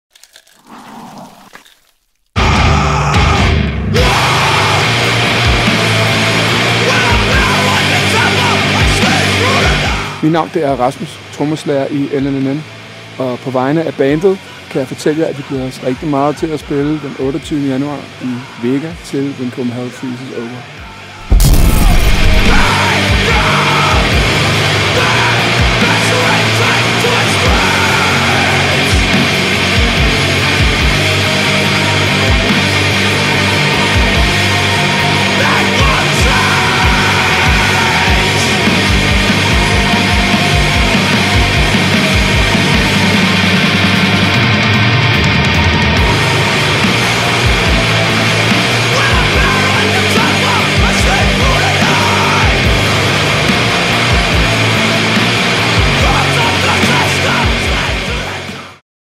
When Copenhell Freezes Over 2017: LLNN
Lørdag den 28. januar blænder When Copenhell Freezes Over igen op for en potent håndfuld spirende, danske metalbands. Et af de bands, du kan opleve, er LLNN.

Live photos in courtesy of Andrei M